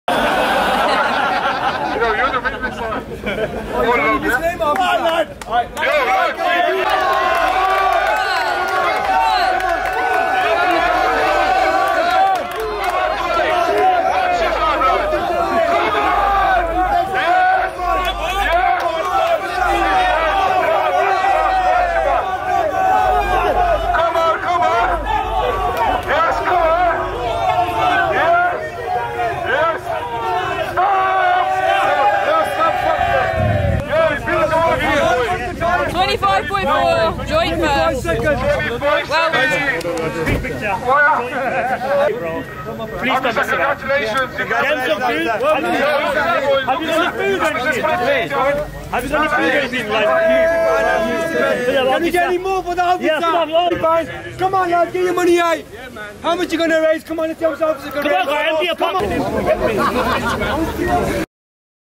POLICE PULL G-WAGON @ MSL CHARITY MEET 2K19
Team MSL hosts their anual charity meet this year & local police officers get invovled rather than cause commotion. This West Midlands Police Officer helped raised over £200 for our cause (in support of mental health uk, Kashmir & build a well Bangladesh) by competing in our Strongman Competion! MASSIVE SHOUT OUT & RESPECT TO THE OFFICER!